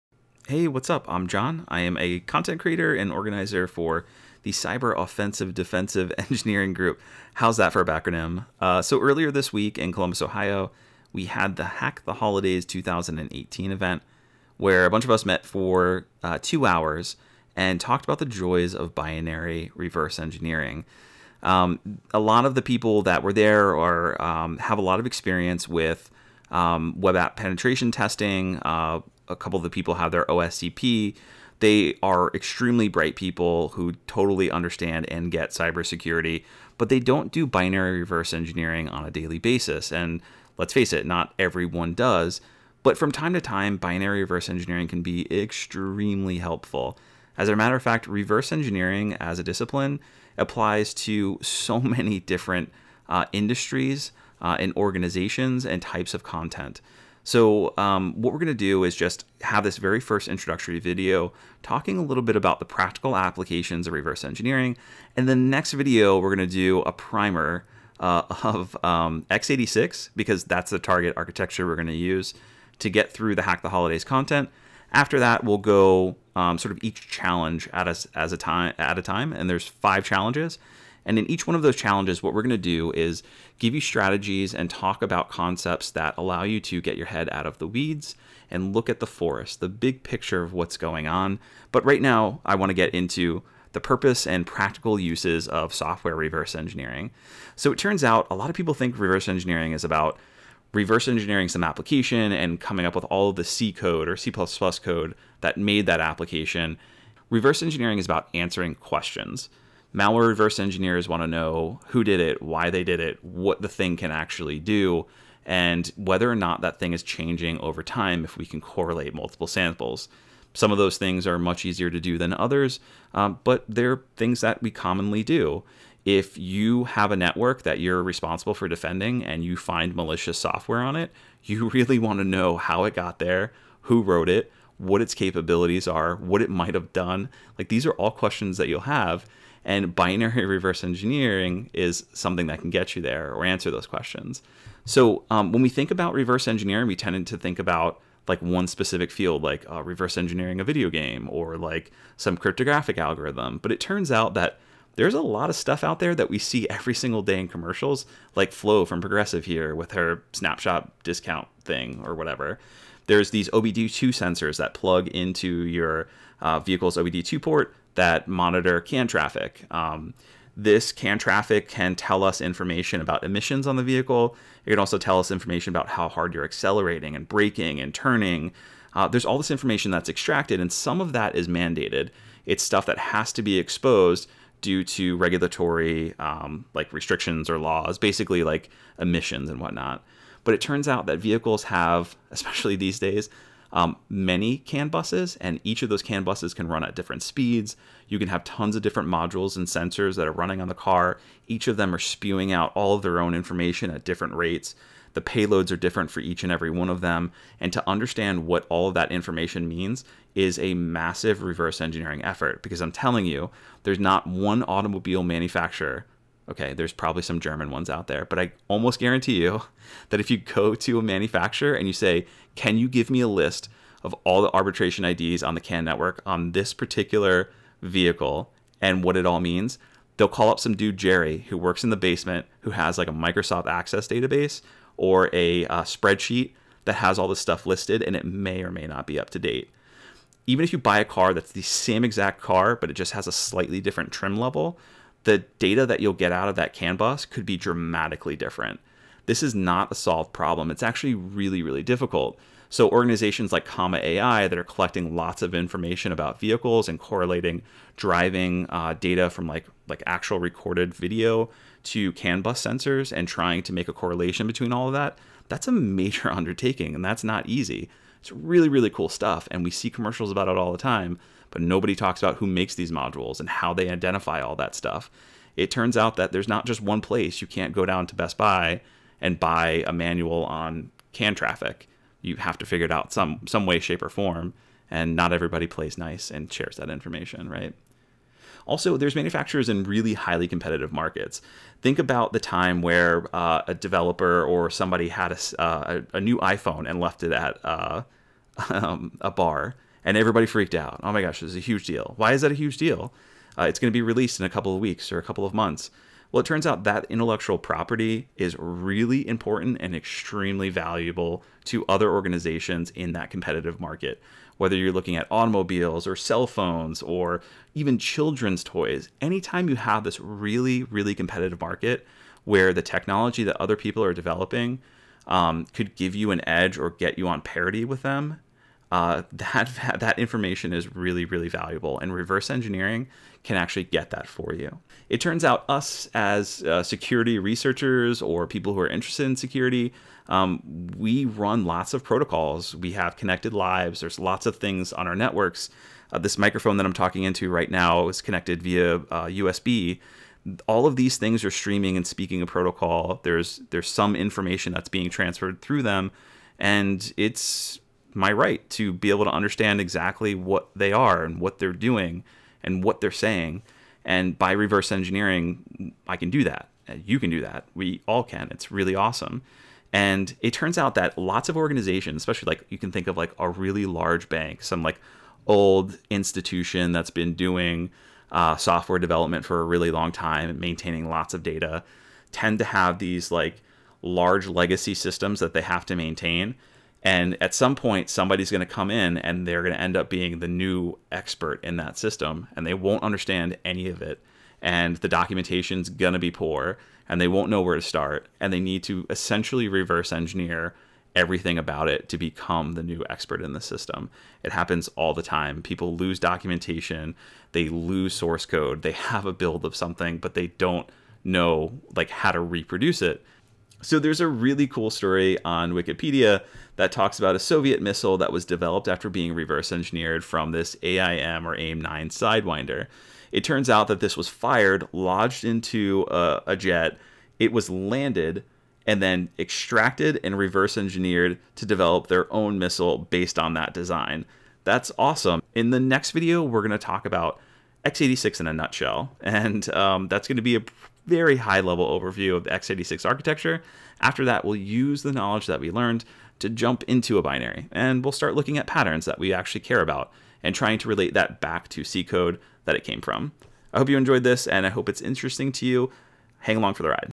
Binary Reverse Engineering: Purpose & Practical Uses of Reverse Engineering (1/8)
From CO/DE: The Joys of Binary Reverse Engineering, Identifying Patterns in Binary Applications